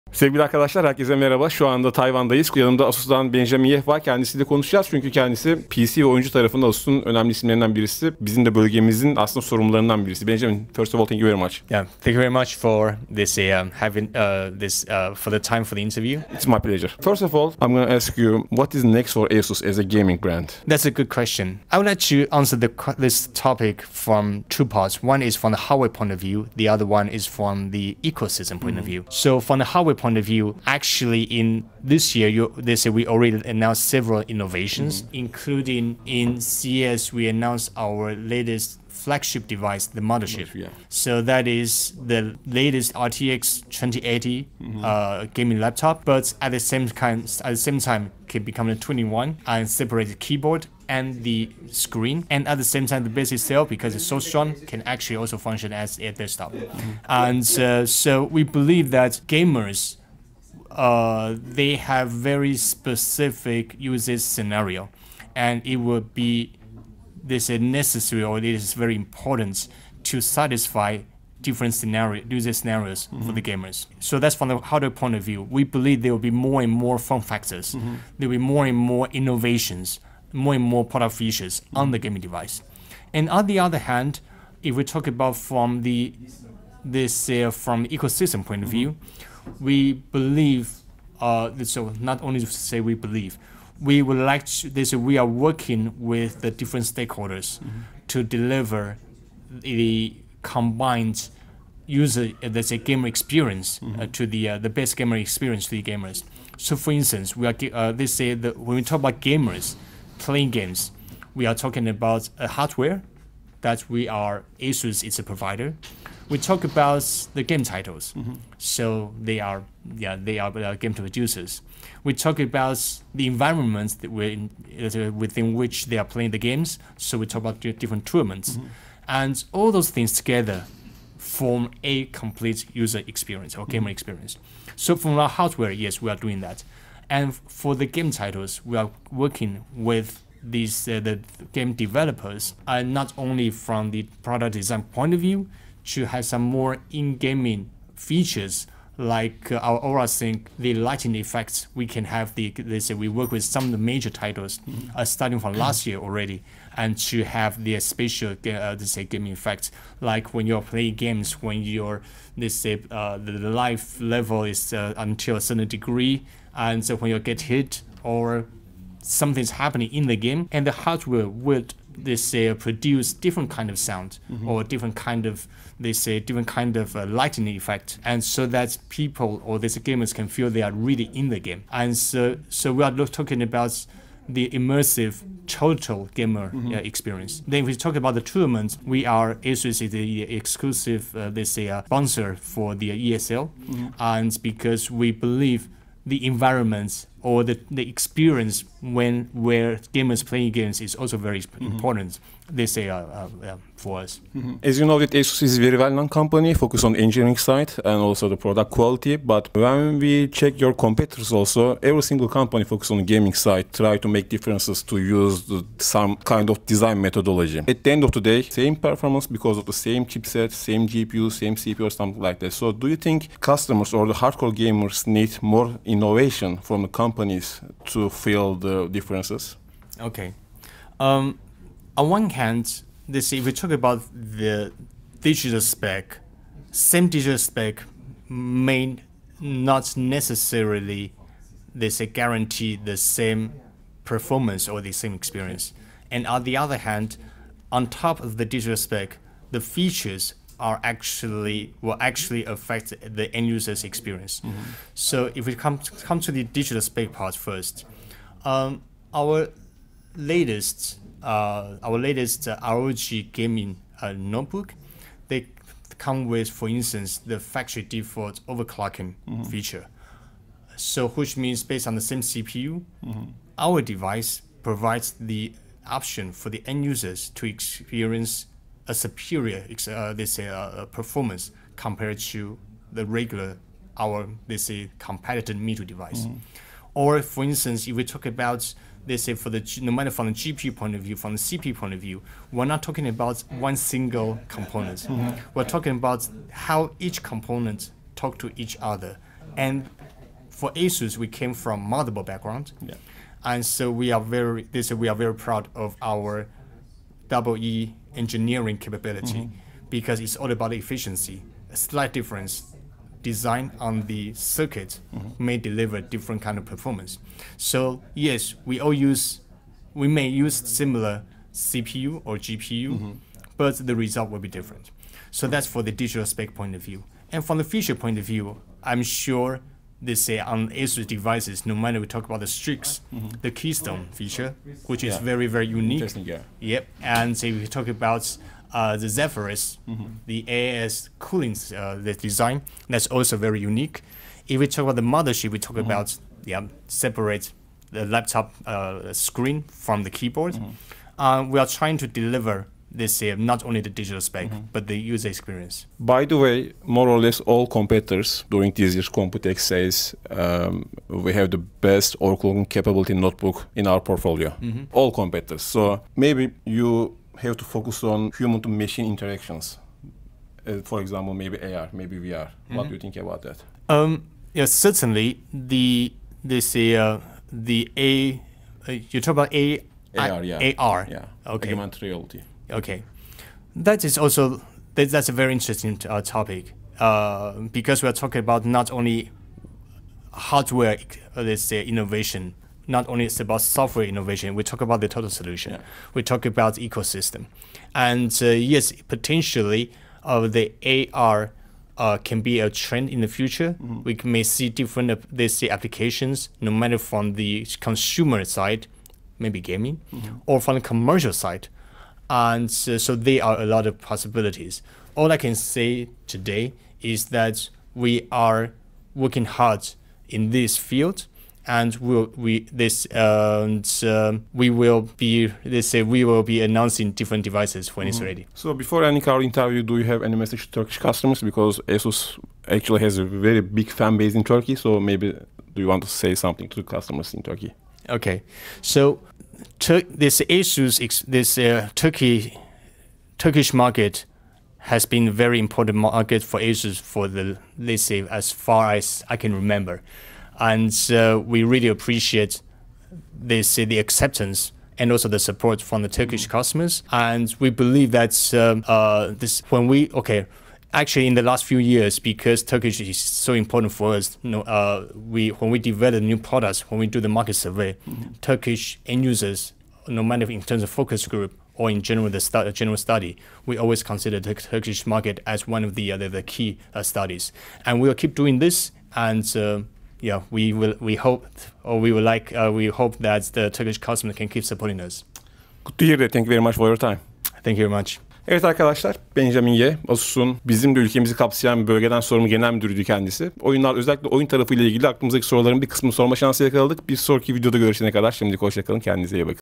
Aynı işlemci ve ekran kartı ile bir bilgisayar nasıl farklılaşabilir?
Asus'un ülkemizi de kapsayan bölgedeki genel müdürü Benjamin Yeh, ile oyun odaklı dizüstü bilgisayarlar üzerine konuştuk. Aynı işlemci ve ekran kartını kullanan dizüstü bilgisayarlar arasında nasıl fark yaratılabilir bunları konuştuk.

Türkiye'de teknoloji forumu denince 1999'dan beri ilk akla gelen portal donanimhaber.com'un YouTube sayfasında teknolojiyle ilgili her şeyi bulabilirsiniz.